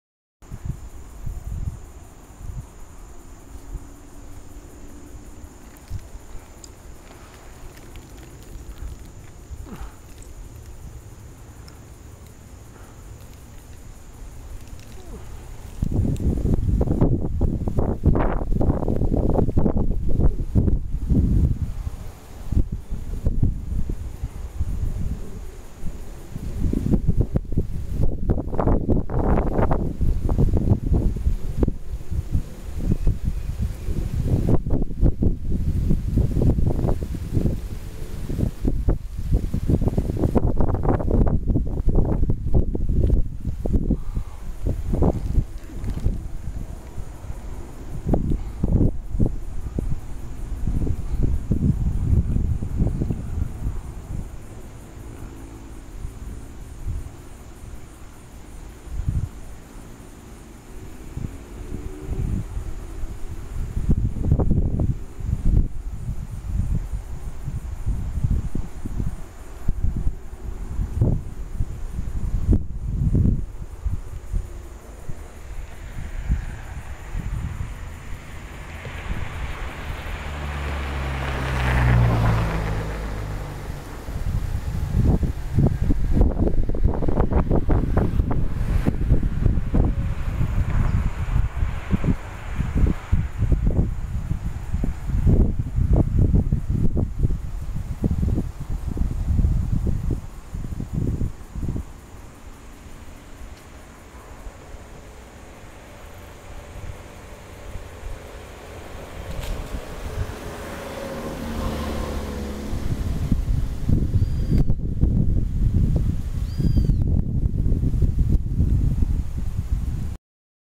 Espantalho na roça , plantação de alimentos.
em   uma    roça  ,  plantação   de  verduras   e    folhas   alface  ,colve ,cebola    ,repolho    etç   colocaram    um  espantalho   para   assustar    os  pássaros  .